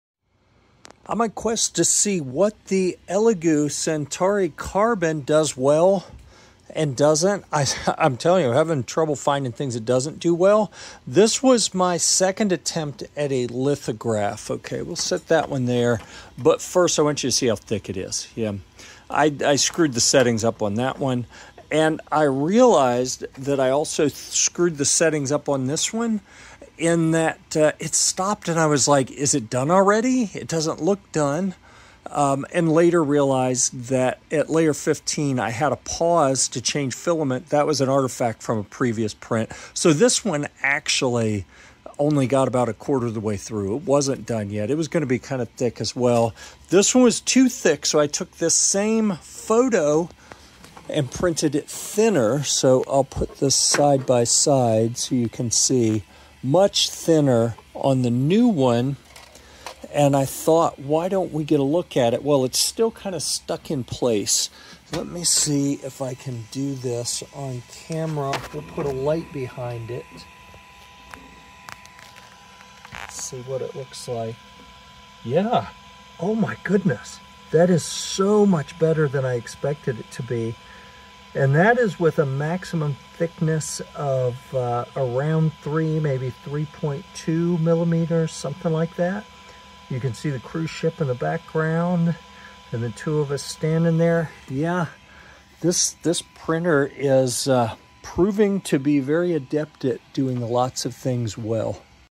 Elegoo Centauri Carbon: What doesn't it do well?
Witness the Elegoo Centauri Carbon in action! Watch us create stunning lithographs.

- SUNLU Official Filament Dryer Box S4,Four-Spool 3D Printer Dehydrator with Rapid Heating, Auto Humidity Control, and Advanced Triple Fan